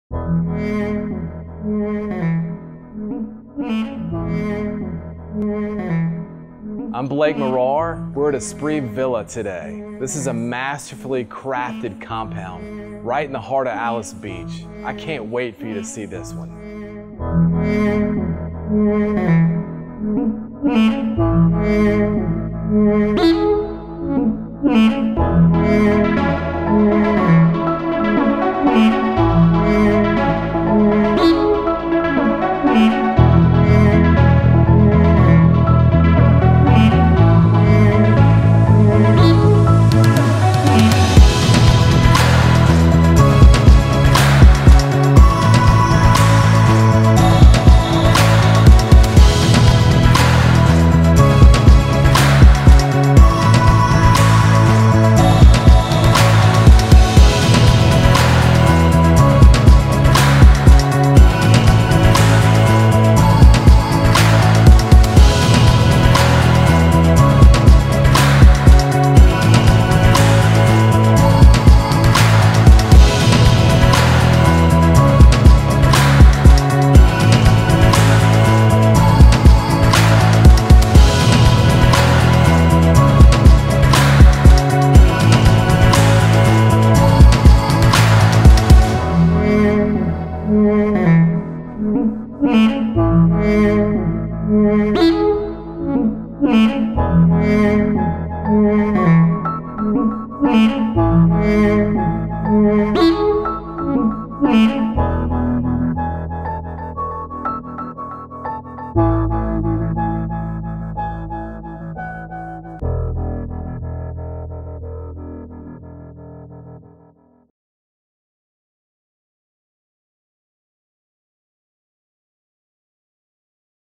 Aspri Villa - 140 North Charles Street
The delicately crafted spaces of this compound home, located in the heart of Alys Beach, were built to embrace reunions of family and friends, or a secluded getaway for two. Envisioned as a true, one-of-a-kind masterpiece, the enchanting Aspri Villa certainly delivers via the skill of renowned Florida architect Tom Christ.